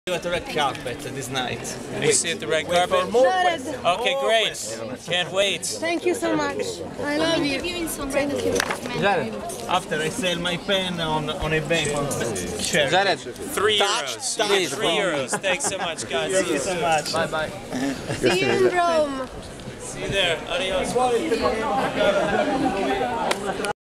Jared Leto fa degli autografi dopo la conferenza stampa - Festival Internazionale del Film di Roma
Jared Leto fa degli autografi dopo la conferenza stampa - Festival Internazionale del Film di Roma 09.11.13